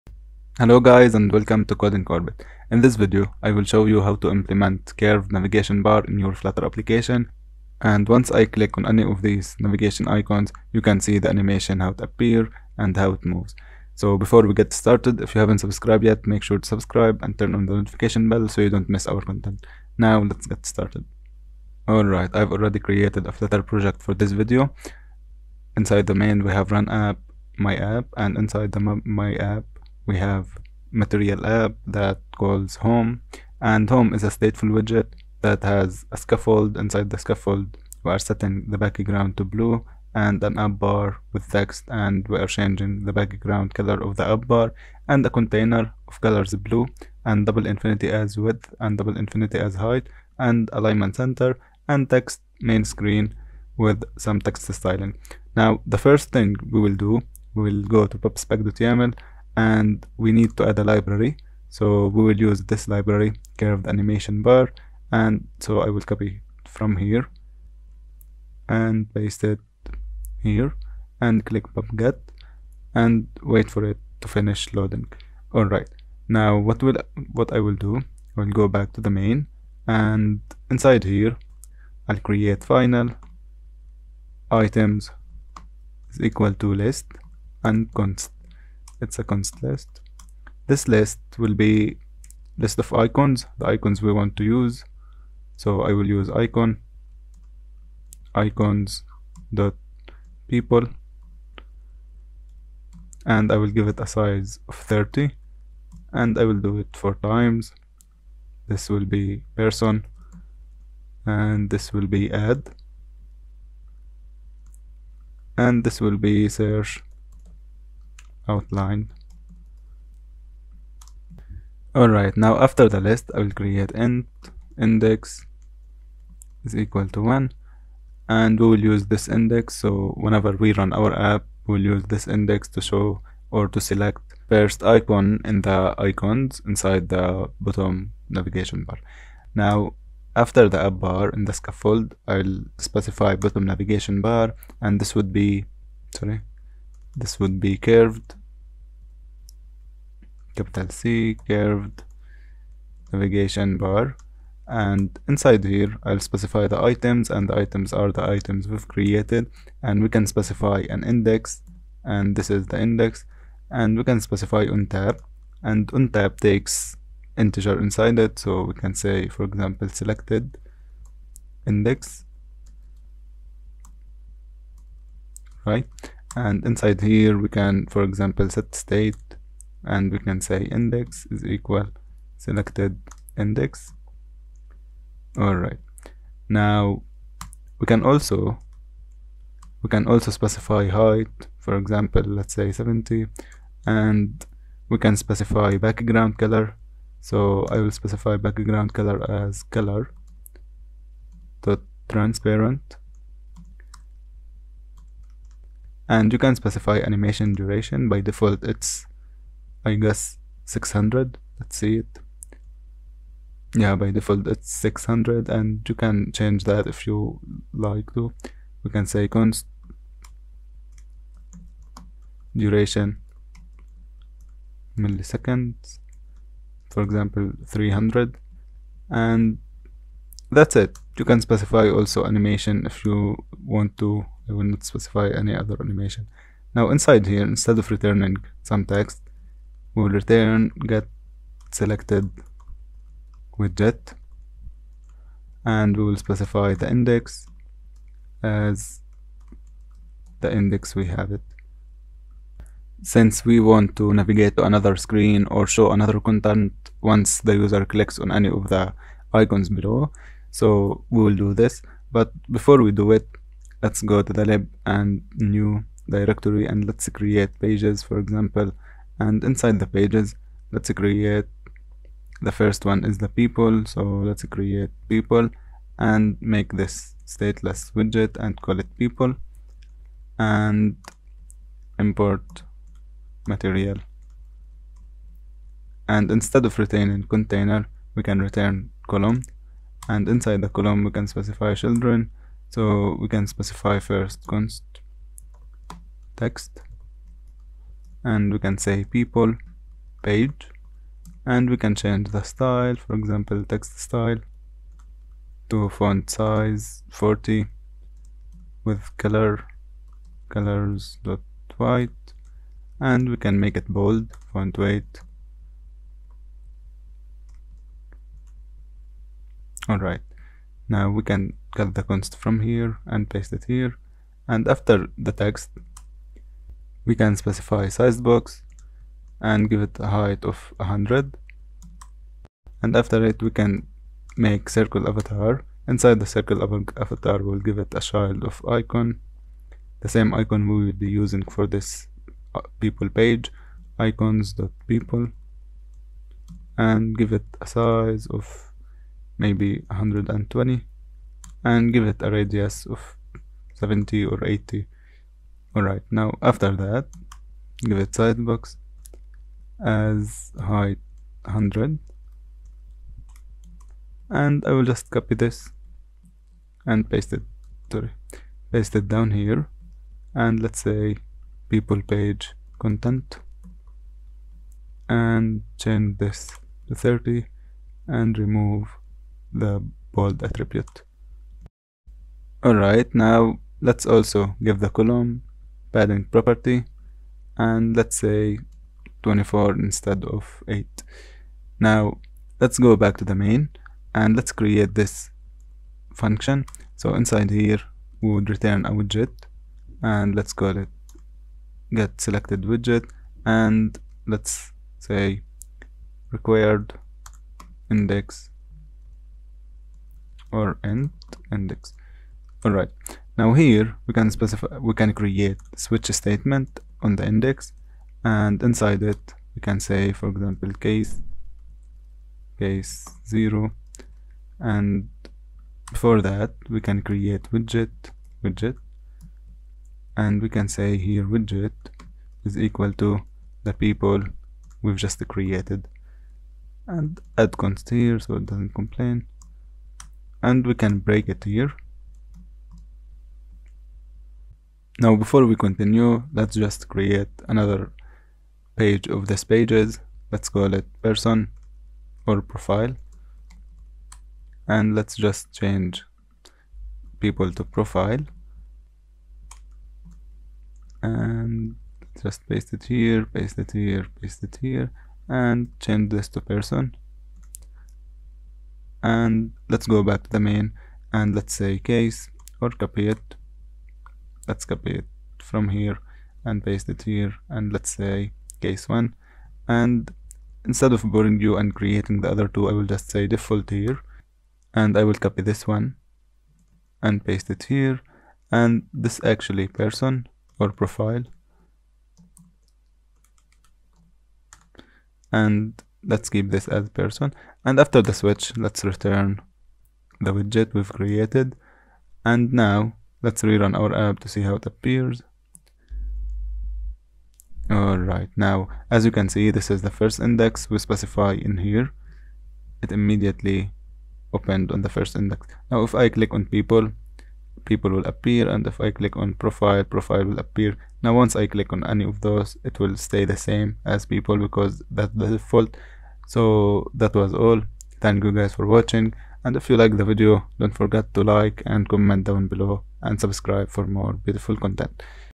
flutter Curved Bottom Navigation Bar
In this Fluter video, you will learn how to add bottom navigation bar in flutter and not any design, you will learn how to implement the curved bottom navigation bar with awesome transition navigation animation.